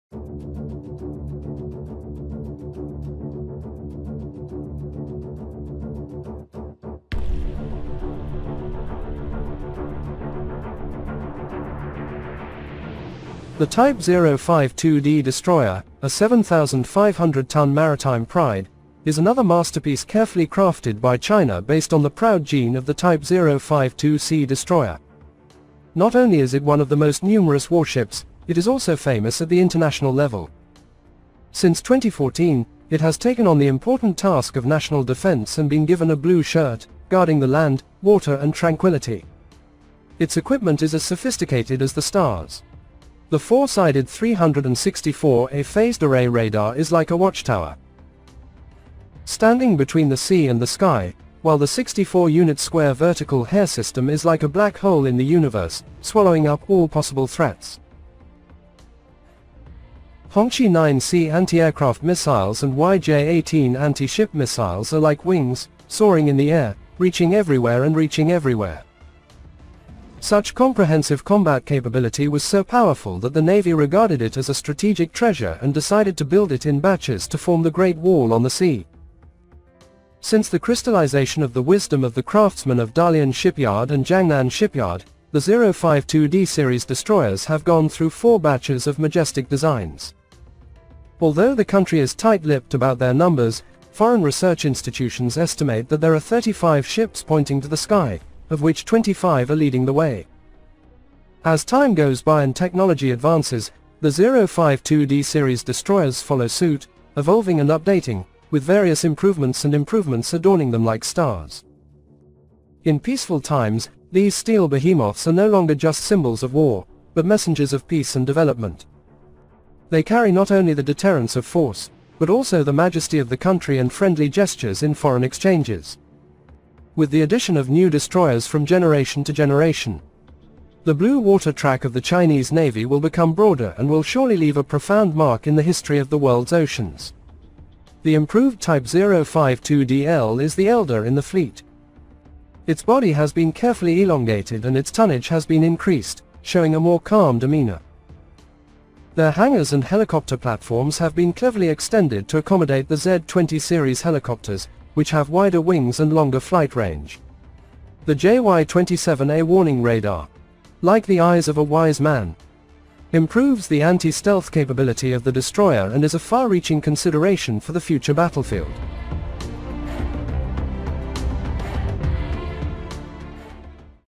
China Type 052D destroyer Upgraded, the latest modification replaces the radar
The Type 052D destroyer a 7.500 ton maritime pride, is another masterpiece carefully crafted by China based on the proud gene of the Type 052C destroyer. Since the crystallization of the wisdom of the craftsmen of Dalian Shipyard and Jiangnan Shipyard, the 052D series destroyers have gone through four batches of majestic designs.